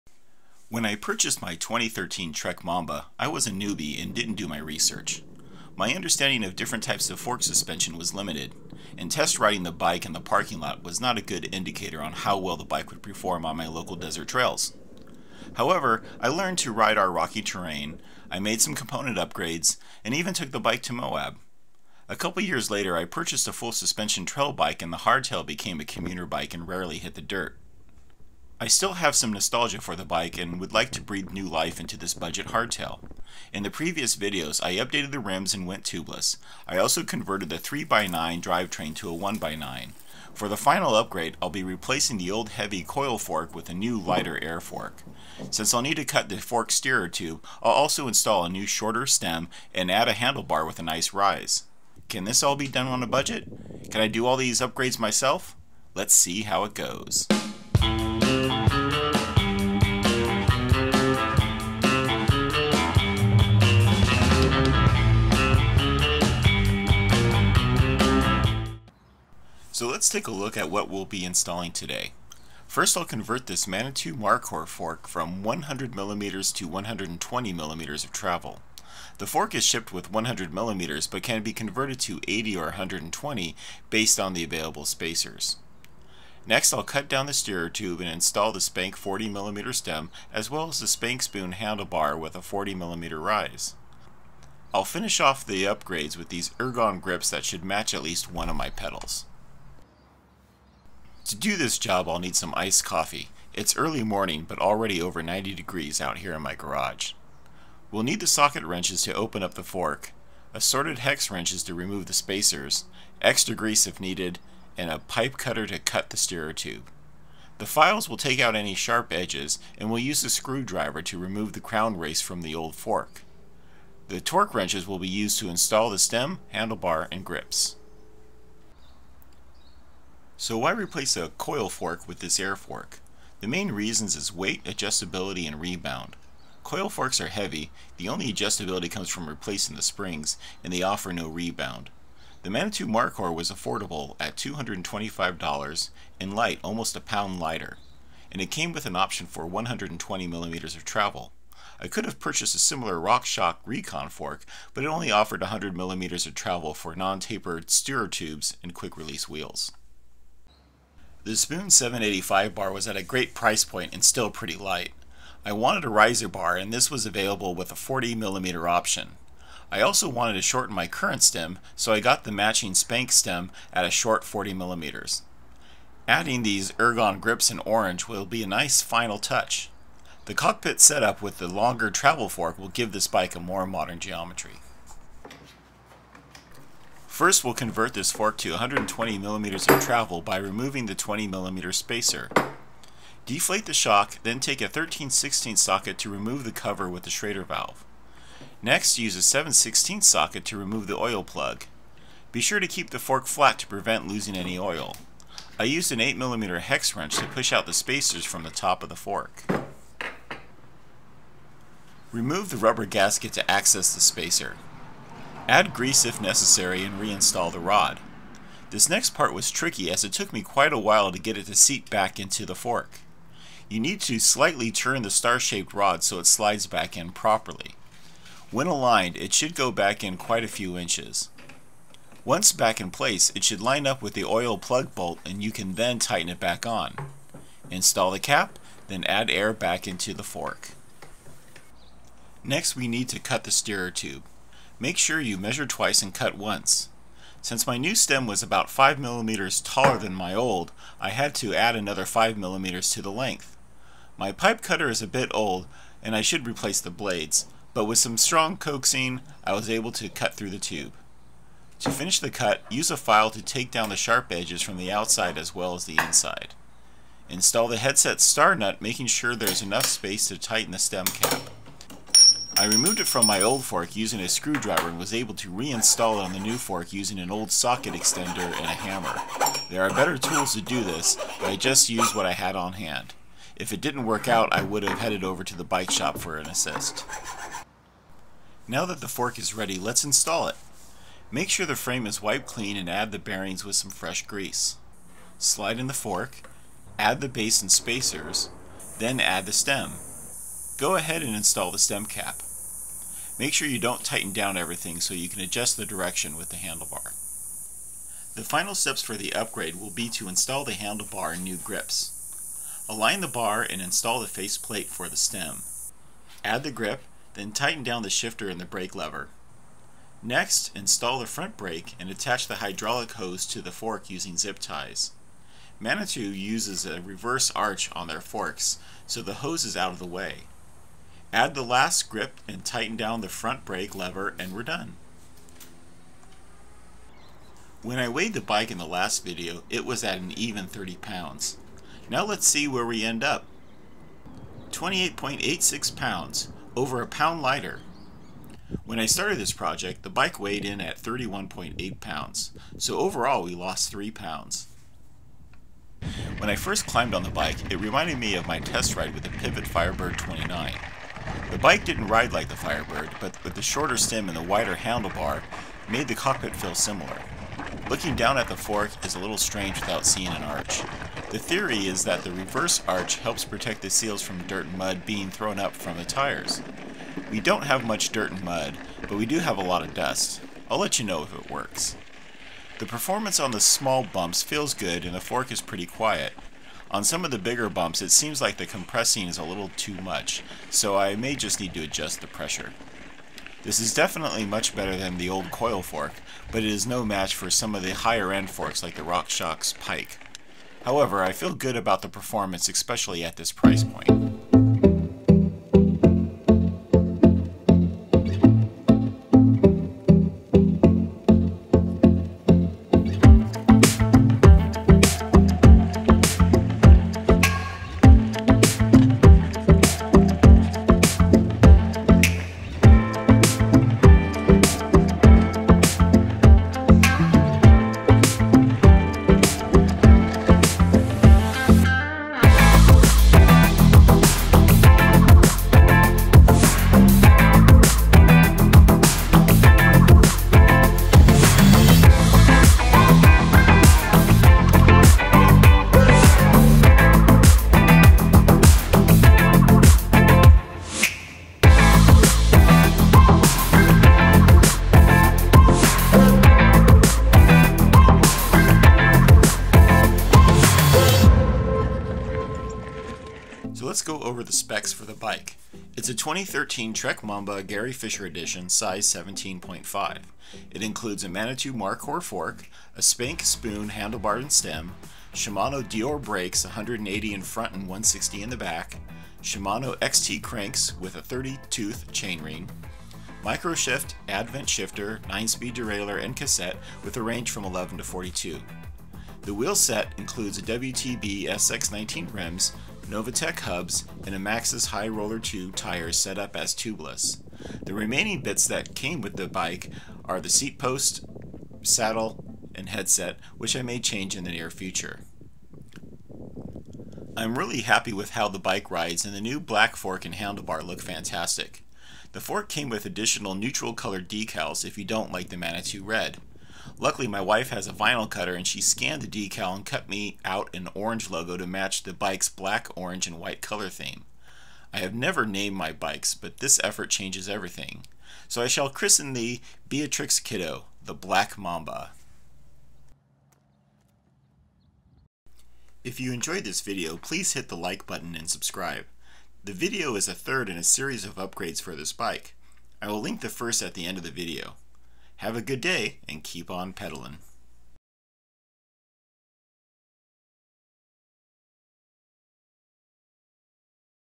🔧 E2.16 New Fork New Life: Installing the Manitou Markhor Fork, Spank Spoon Handlebar and Stem
Fred does his last upgrades to his hardtail installing a new air fork, handlebar and stem. The fork suspension is increased to 120mm and the stem is shortened. With the riser handlebar will it feel more like a modern bike?

Parts

Spank Spoon 2 Stem - 40mm, 31.8mm, 0 Degree, Alloy, Black